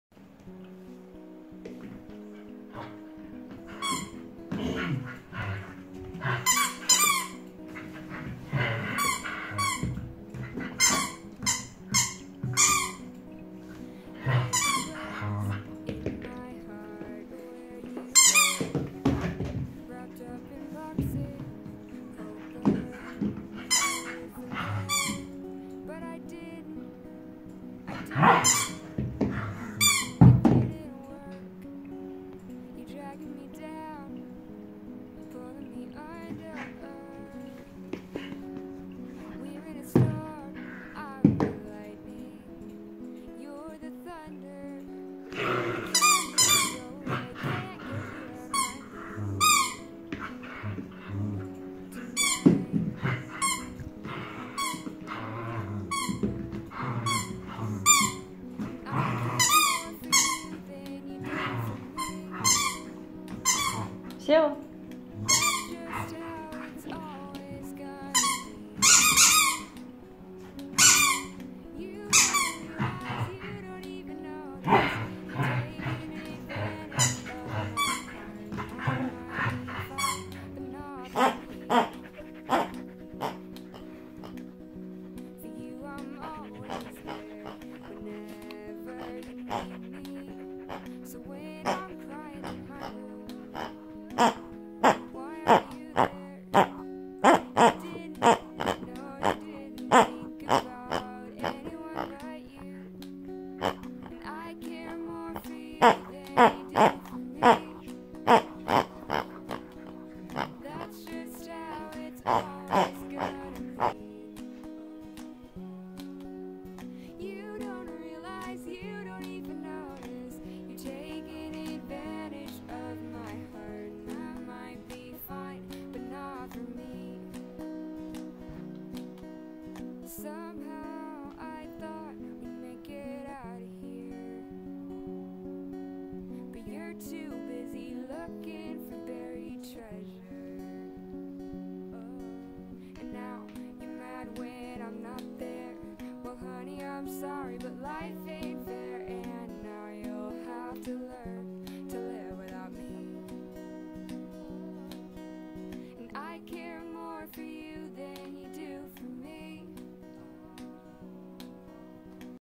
Шелти играет🦊🎮 | Любимые игрушки 🤡
Собака породы шелти Гривус, 2 годика, забавные игры
Игрушки для собак